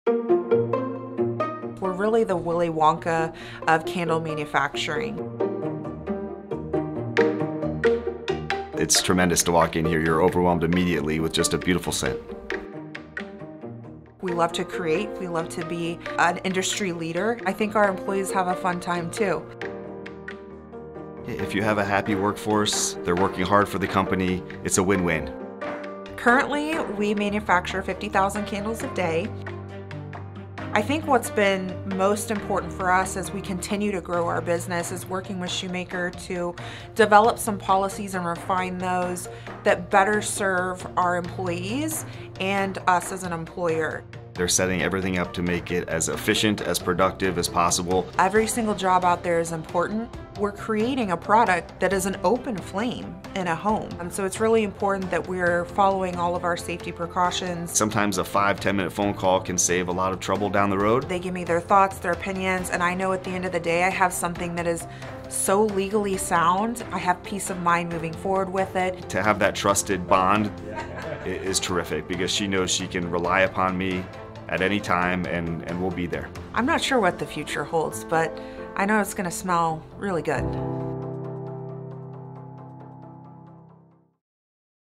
Shumaker: Scentsational Testimonial
Hear how Shumaker is a trusted partner to Scentsational Soaps and Candles, giving peace of mind through legally sound policies that better serve both the employees and the employer.